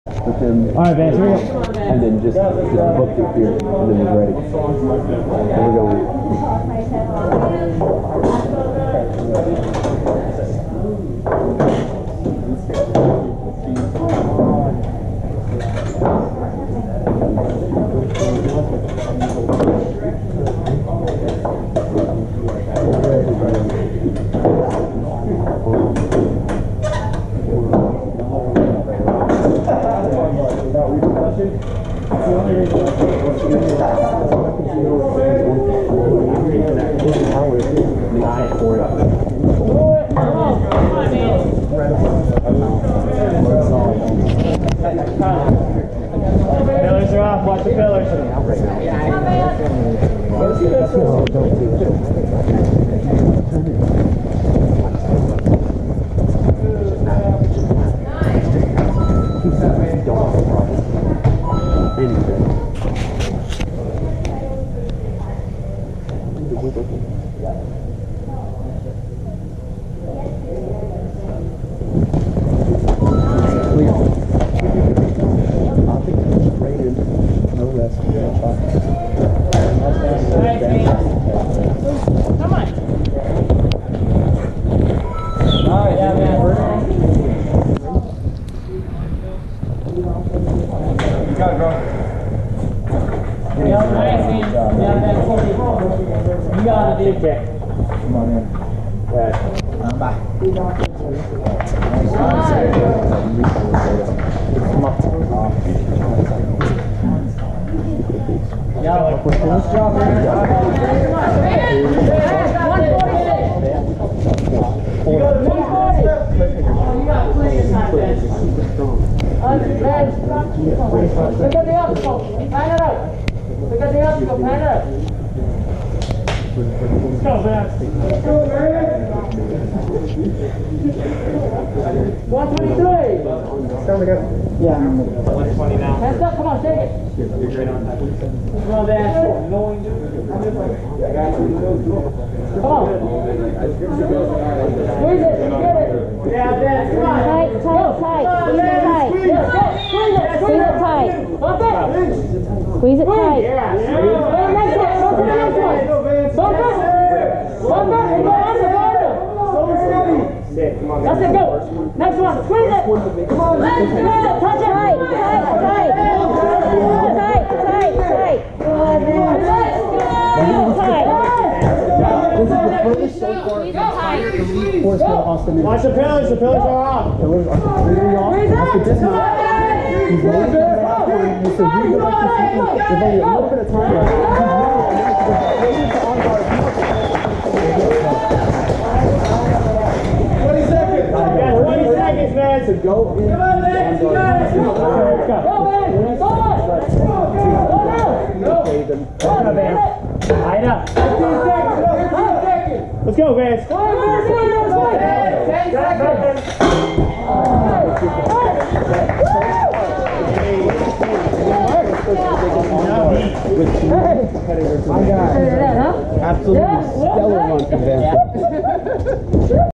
Vance Walker 1st Place Austin Ninjas Cedar Park (#2) | National Ninja League Season 6
Vance Walker goes the distance on one of the toughest courses of the season, finishing two obstacles clear of one of the most loaded fields in recent memory.